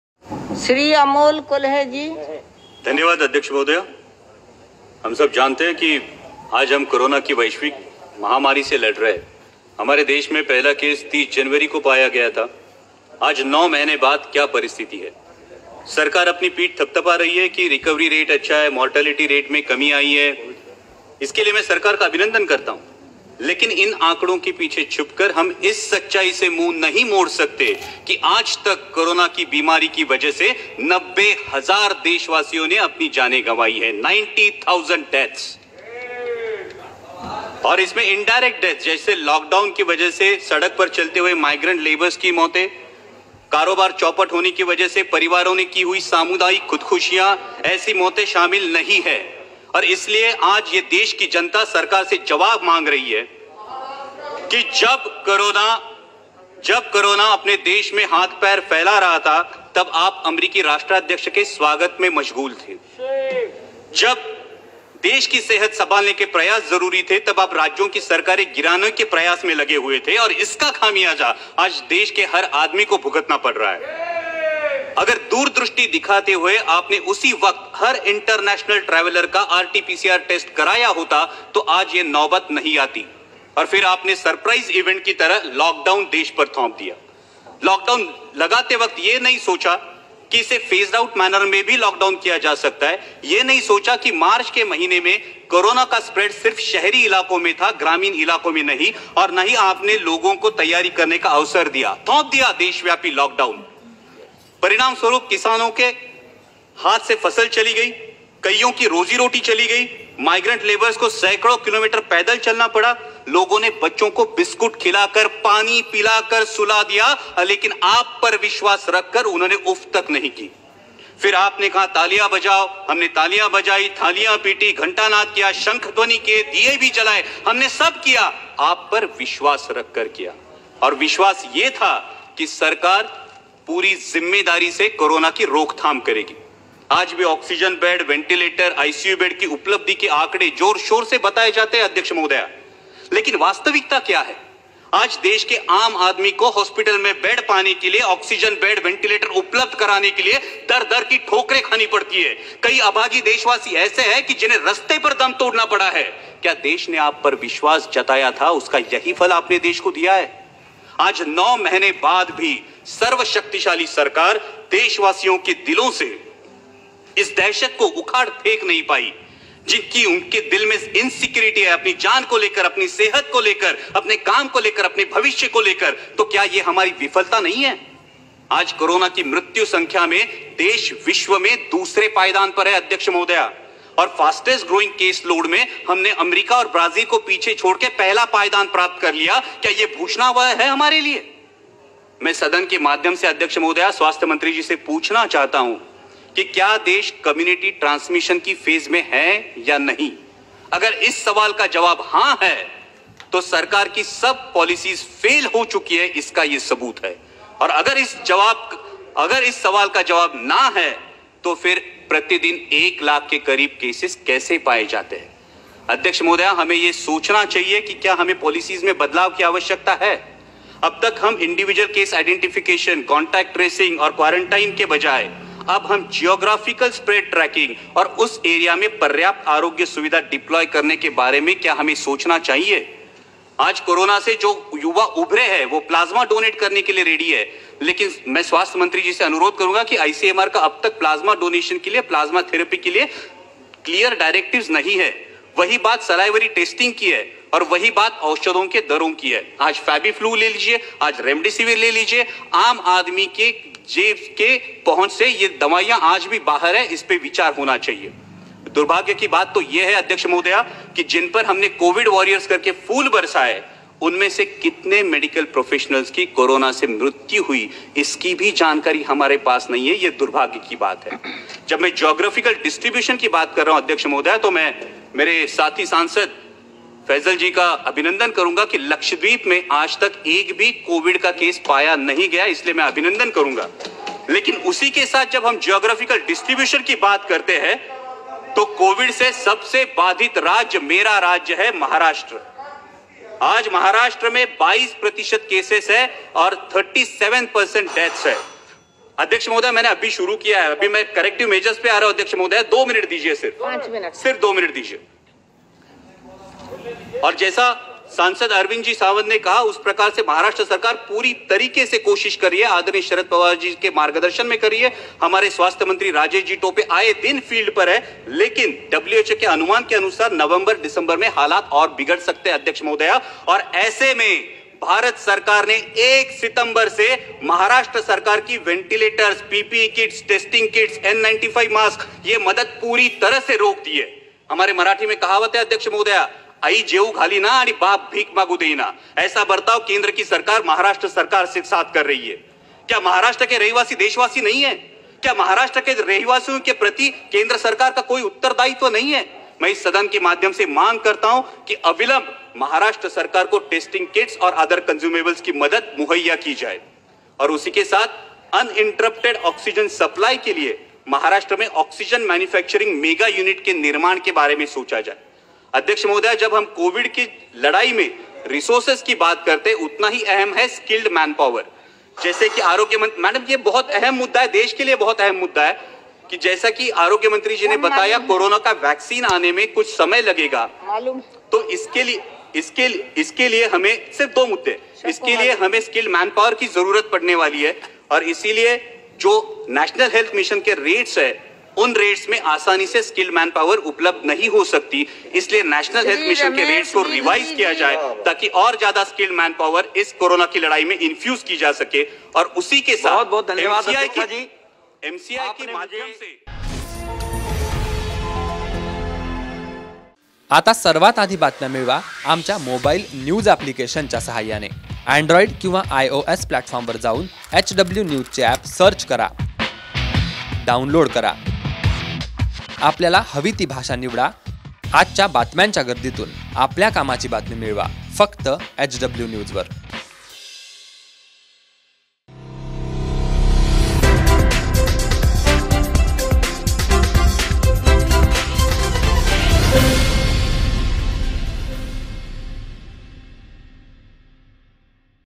‘महाराष्ट्रावर केंद्राने अन्याय केला‘ अमोल कोल्हे लोकसभेत कडाडले ...हे भाषण पाहाचं! | Amolkolhe
केंद्रानं महाराष्ट्राकडे अधिक लक्ष द्यायला हवं. मात्र त्याउलट १ सप्टेंबर पासून केंद्र सरकारने महाराष्ट्राची व्हेंटीलेटर्स, पीपीई किट, टेस्टींग किट, एन ९५ मास्क आदींची मदत पुर्णपणे थांबविली आहे. ही अत्यंत चिंतेची बाब असल्याचे सांगत 'आई जेवू घालीना आणि बाप भीक मागू देईना' अशी वागणूक केंद्र सरकारकडून महाराष्ट्राला मिळत आहे, हे खासदार अमोल कोल्हेंनी  सभागृहाच्या निदर्शनास आणून दिले.